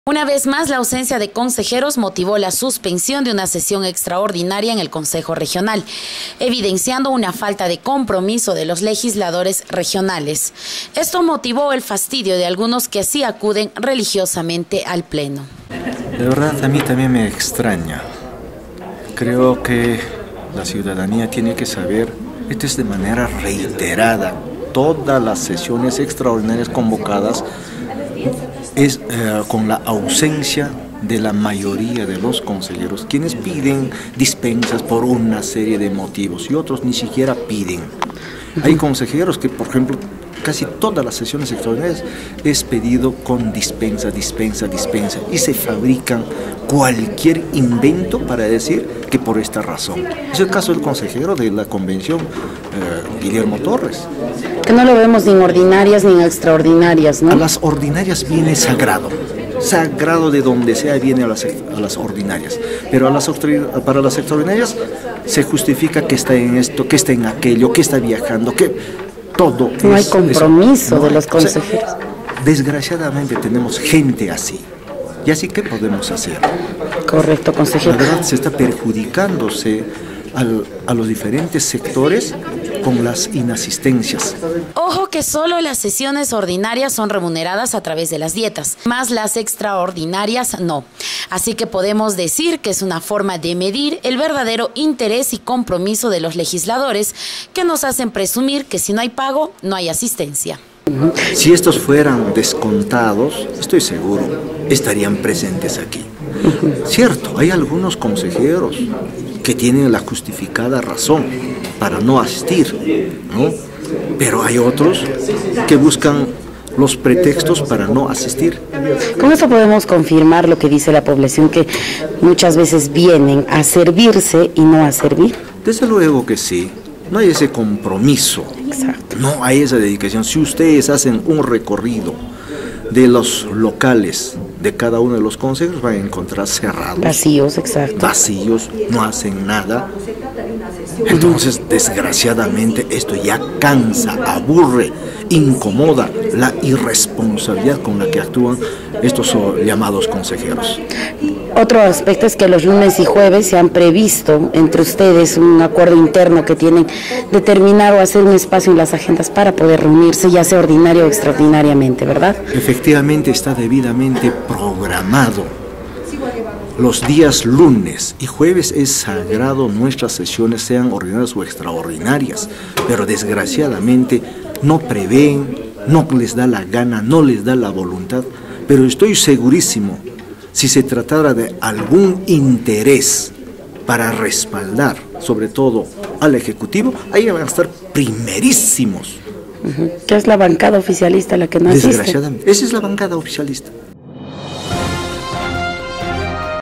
08 setiembre 2016 VICTOR VARGAS SANTANDER CONSEJERO REGIONAL DE PAUCARTAMBO DEL CUSCO
POR ENESIMA VEZ, AUSENCIA DE CONSEJEROS REGIONALES MOTIVÓ LA SUSPENSIÓN DE LA SESIÓN EXTRAORDINARIA … CONSEJERO VÍCTOR VARGAS CRITICO A SUS COLEGAS POR ASISTIR SOLO A LAS SESIONES ORDINARIAS QUE SON PAGADAS …